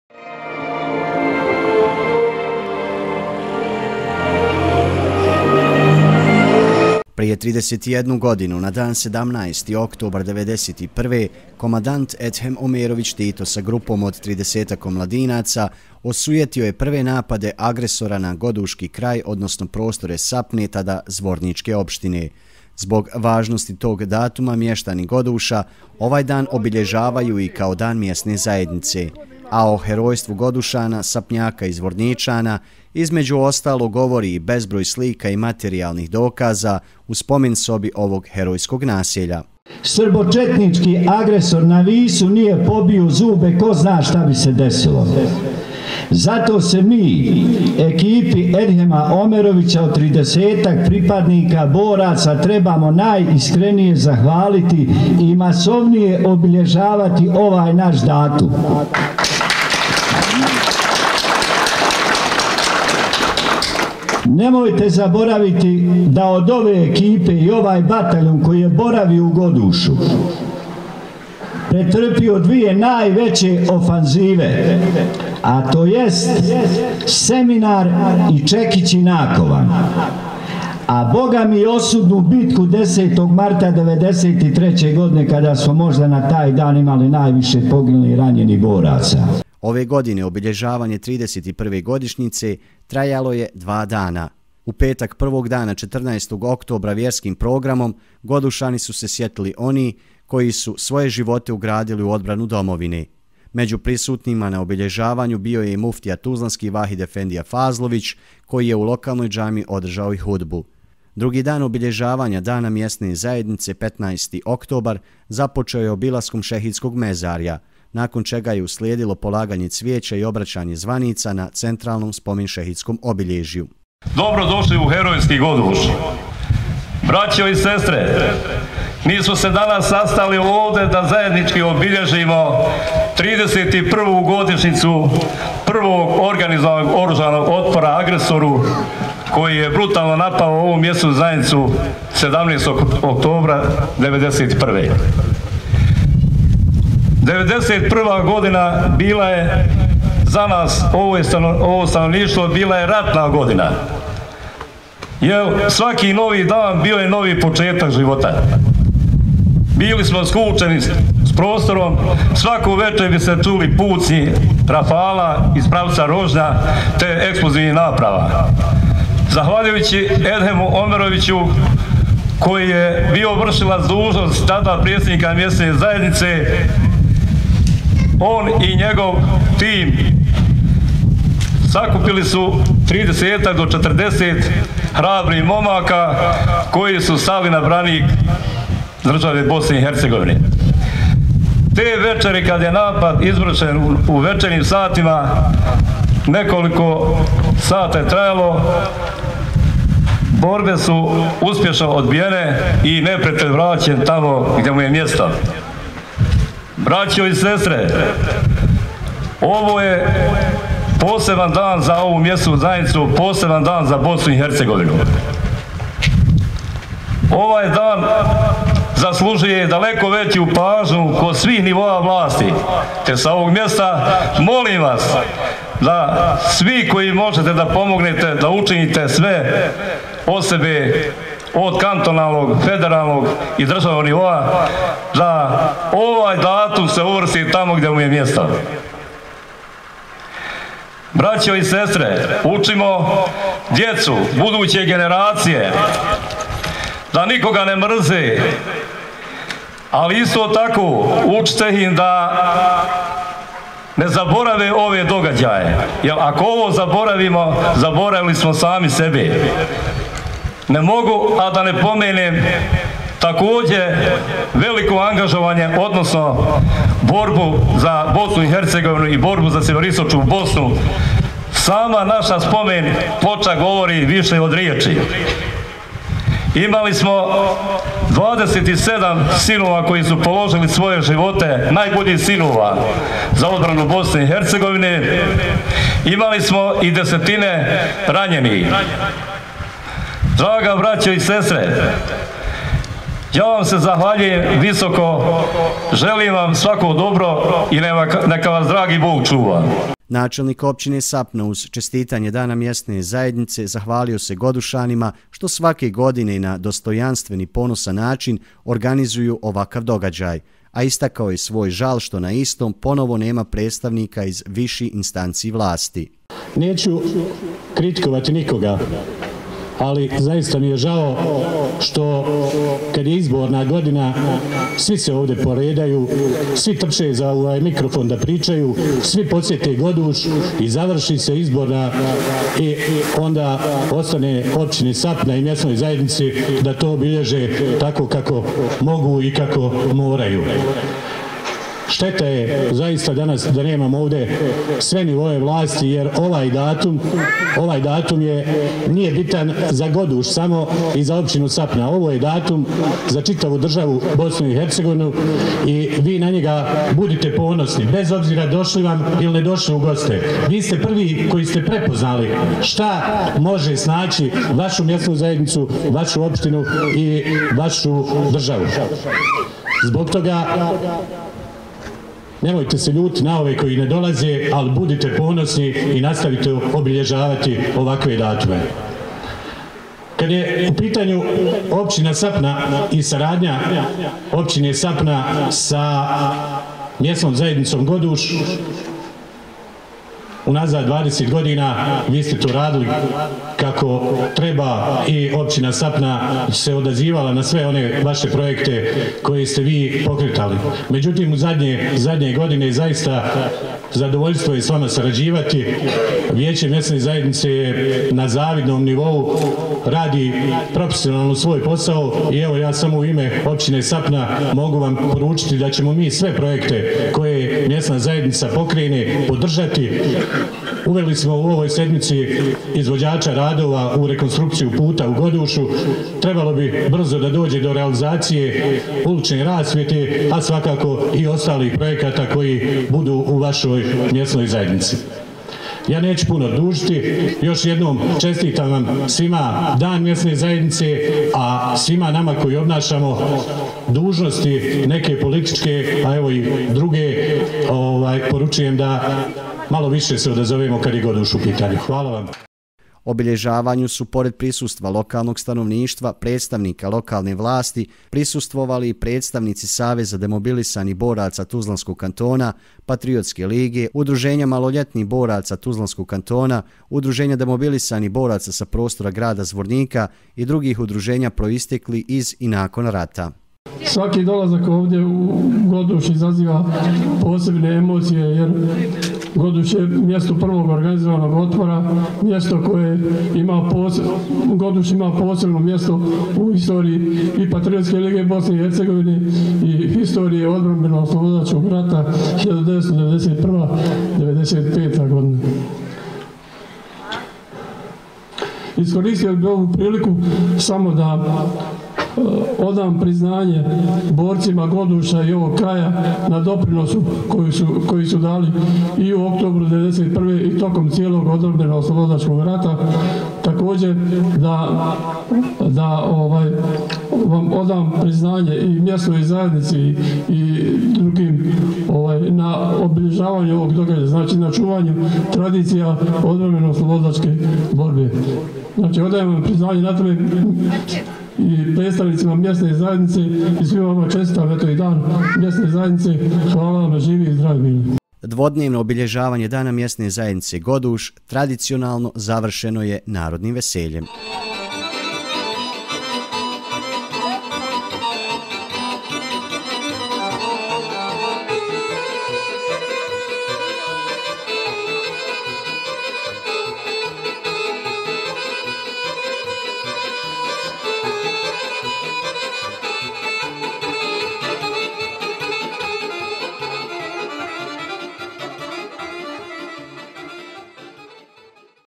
GODUŠ, SAPNA - Prvi organizovani otpor agresoru 1991. godine - [Podrinje Media]
1991. godine srpske paravojne formacije čiji je položaj bio na području Rožnja odlučile su da izvrše napad na Godušu kod Sapne. Tu su ih dočekali goduški branioci. Borba nije trajala dugo, a neprijatelj, iznenađen otporom, bio je primoran da se povuče na početni položaj.